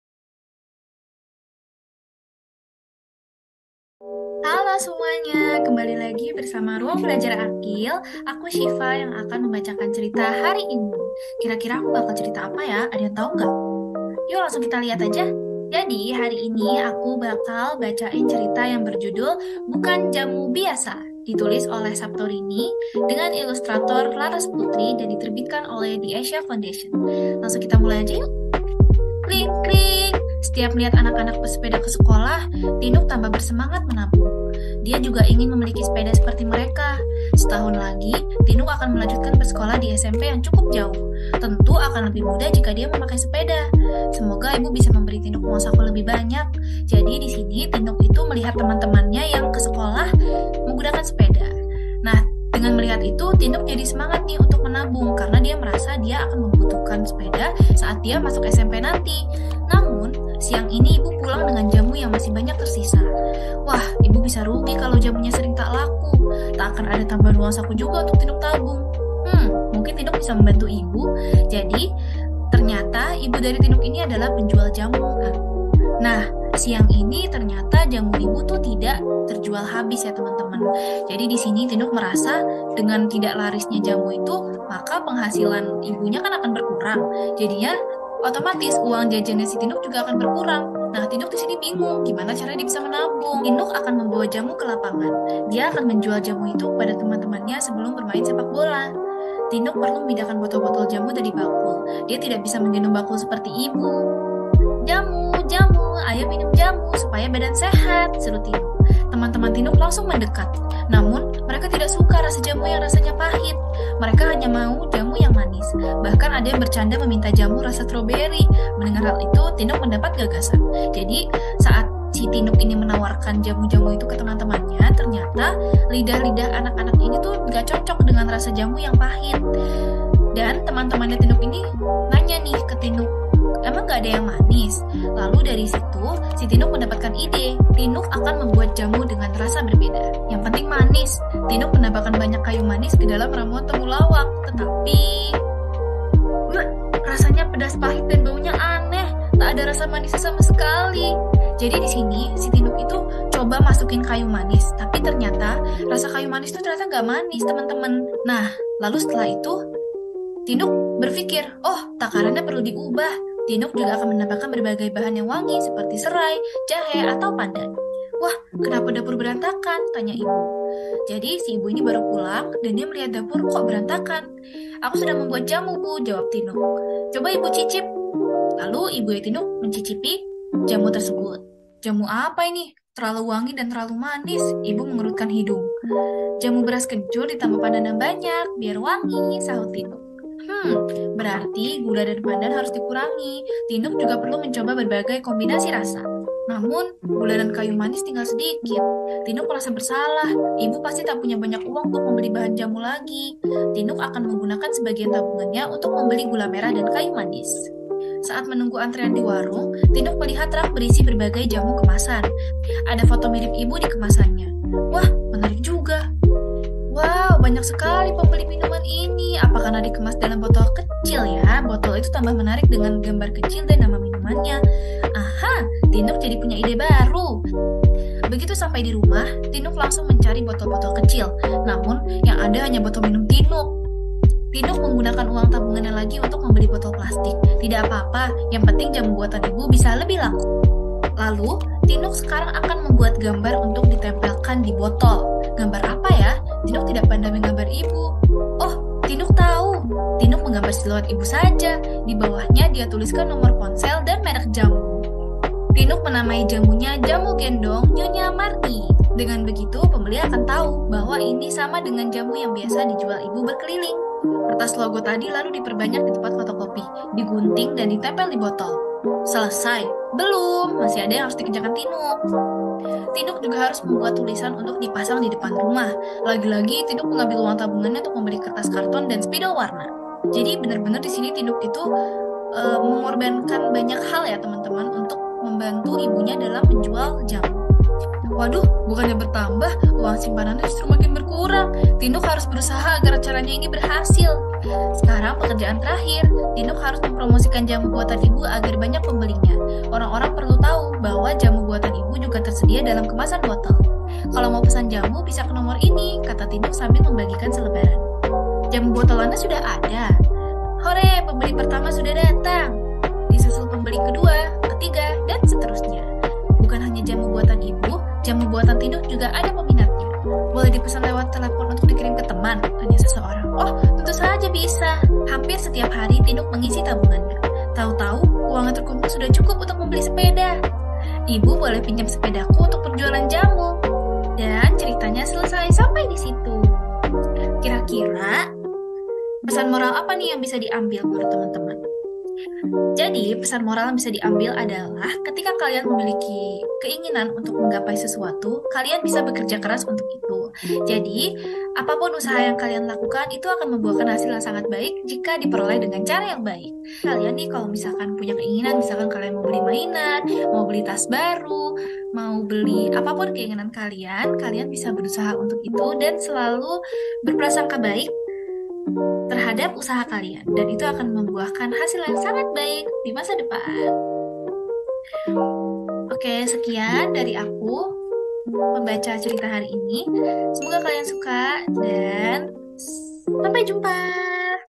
Bukan Jamu Biasa | Read Aloud
"Setahun lagi, Tinuk akan melanjutkan bersekolah di SMP yang cukup jauh. Tentu akan lebih mudah jika dia memakai sepeda. Tinuk berjualan jamu dan keuntungannya ditabung untuk membeli sepeda. Bagaimana perjuangan Tinuk menjual jamu? Bisakah Tinuk melakukannya?

Judul: Bukan Jamu Biasa
Penulis: Saptorini
Ilustrator: Larasputri
Penerbit: The Asia Foundation
Penutur: Hanif Mutiara Syifa

Buku ini diterbitkan di bawah lisensi CC-BY-4.0 dan dituturkan dalam bahasa Indonesia. Segala bentuk perubahan dan improvisasi dilakukan oleh penutur.